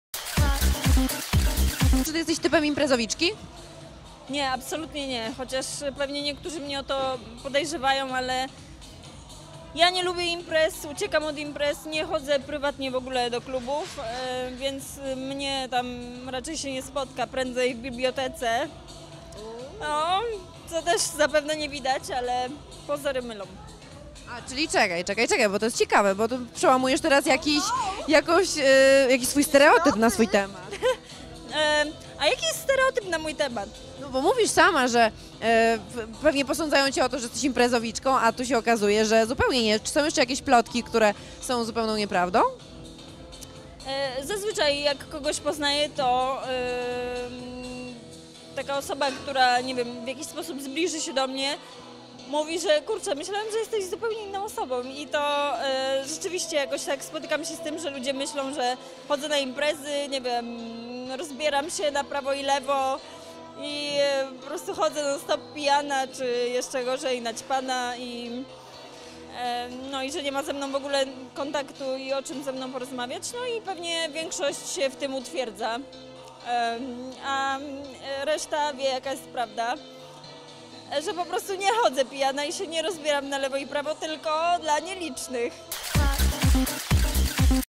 Joanna Majstrak: "Ludzie myślą, że rozbieram się na prawo i lewo"
Joanna Majstrak zasłynęła z pokazaniu biustu na ściance, a także wyznania, że jest biseksualna. Aktorka twierdzi, że w mediach, a także powszechnej świadomości funkcjonuje jako kontrowersyjna skandalista. Tym czasem częściej przebywa w bibliotece niż w nocnych klubach:

Nie jestem typem imprezowiczki. Chociaż niektórzy mnie o to podejrzewają. Ja nie lubię imprez. Uciekam od imprez. Nie chodzę prywatnie w ogóle do klubów.

Ludzie myślą, że chodzę na imprezy i rozbieram się na prawo i lewo. Chodzę non stop pijana, lub jeszcze gorzej naćpana -- dodała gwiazda. Nie chodzę pijana i rozbieram się tylko dla nielicznych.